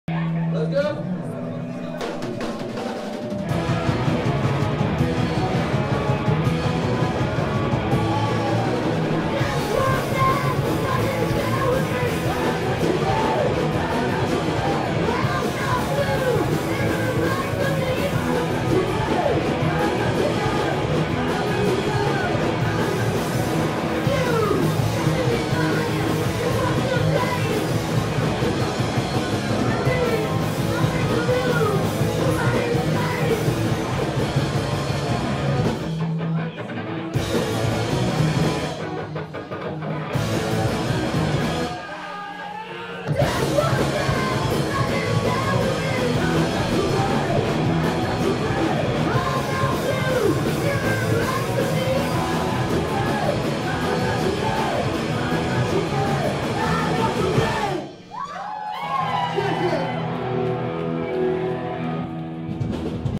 Thee Oops at Temple (Beijing) 11/10/2013
Thee Oops performing at Temple in Beijing in November 2012. For more music out of China's capital go to livebeijingmusic.com